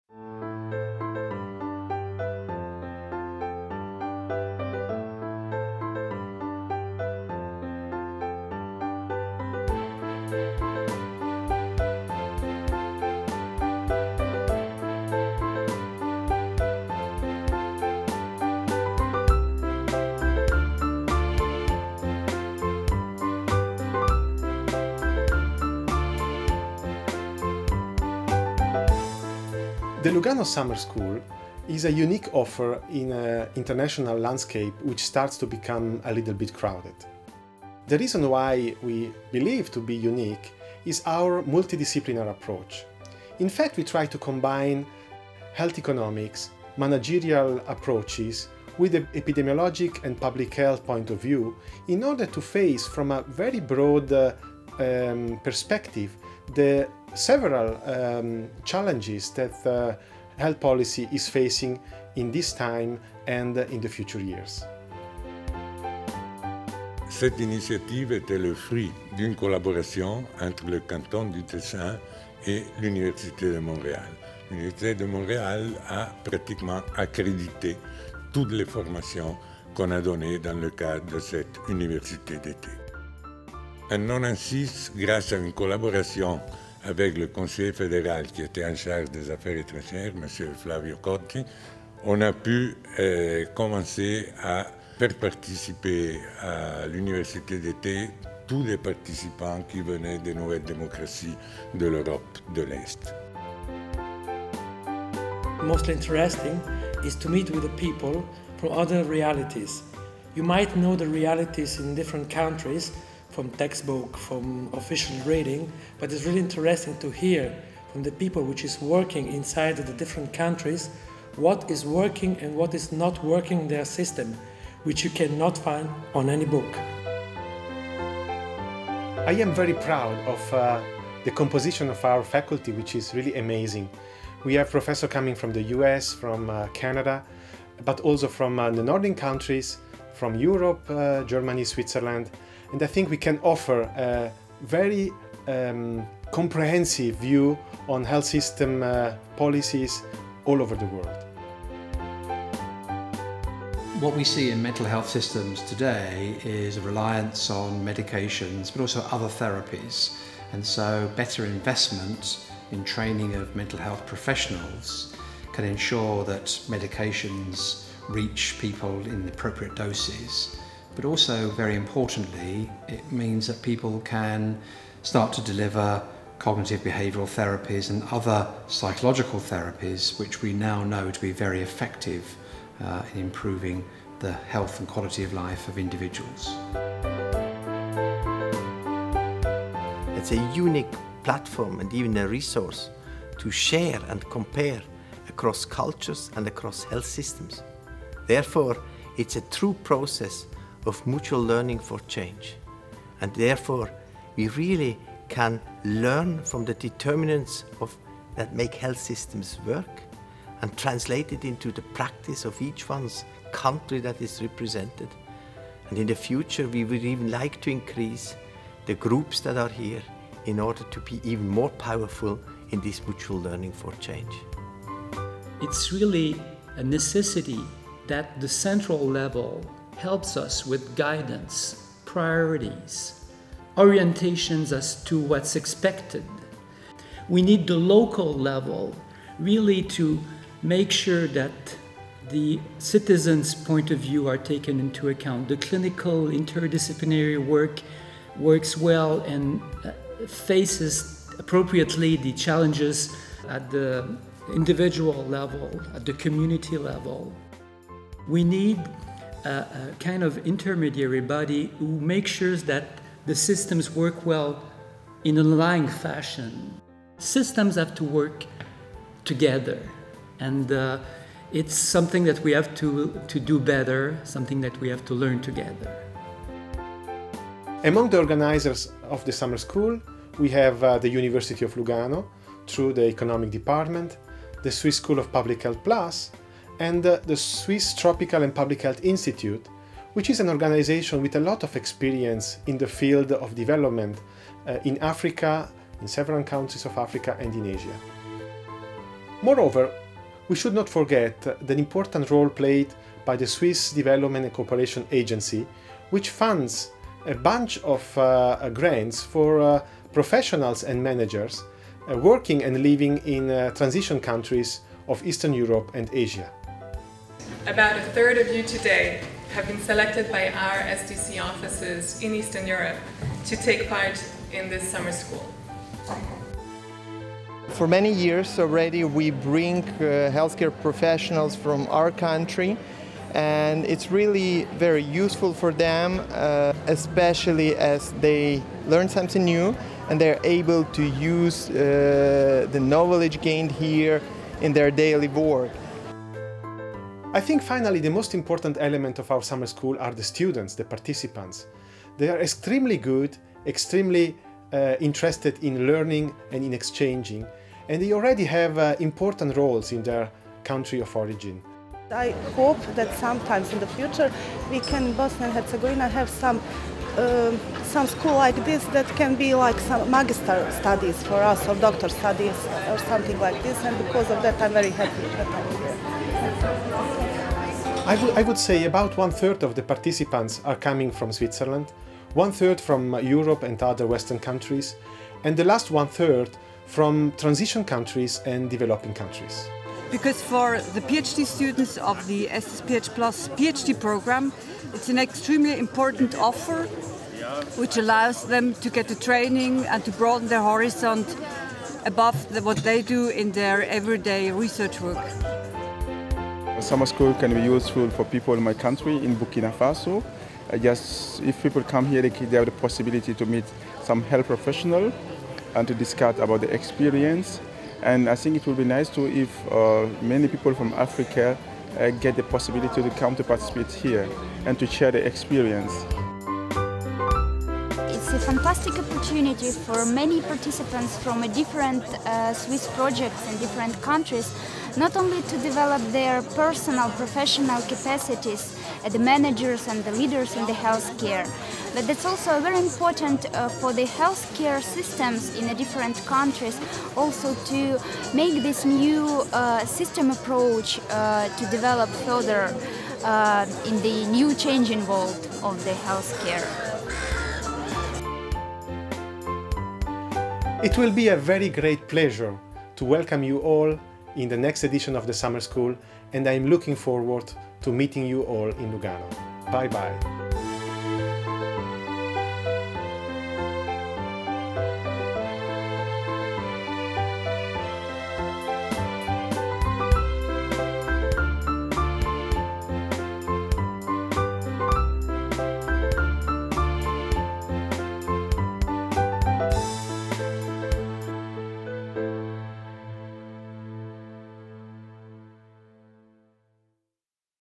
USI Summer School
The Summer School in Public Health Policy, Economics and Management is organised by the Foundation Swiss School of Public Health (SSPH+), the Institute of Economics (IdEP) of the University of Lugano and the Swiss Tropical and Public Health Institute (Swiss TPH).